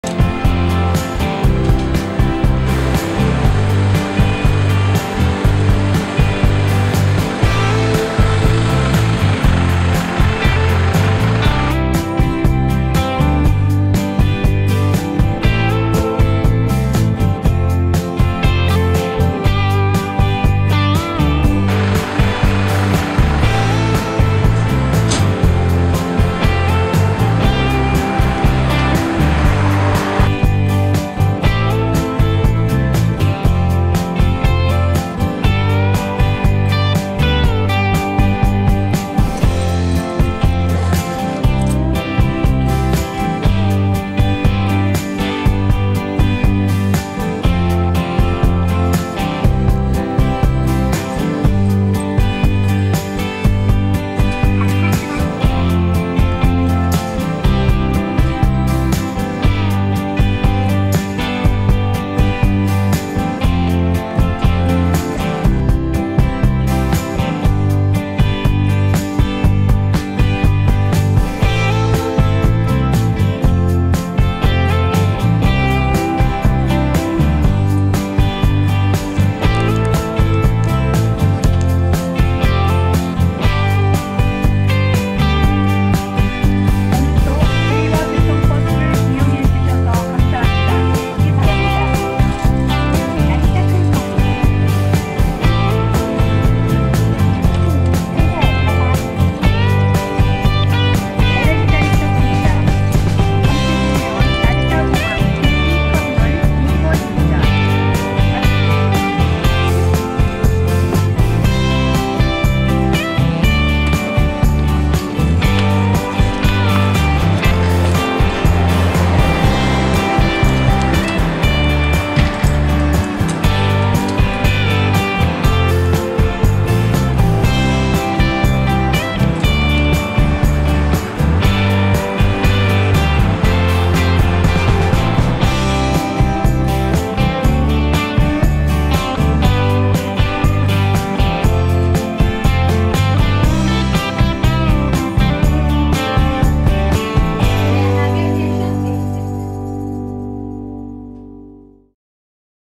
단돈 1,000엔 도쿄에서 나리타공항 리무진버스 타기! (초보필견)
도쿄 긴자역과 도쿄역에서 출발하는 단돈 1,000엔 리무진버스로 나리타공항가기 꿀팁입니다.
[갱신]
2021년3월1일부로 가격변동이 있습니다. 성인 1,300엔.
개정후 승차한 영상은 밑에 링크를 통해 확인하세요.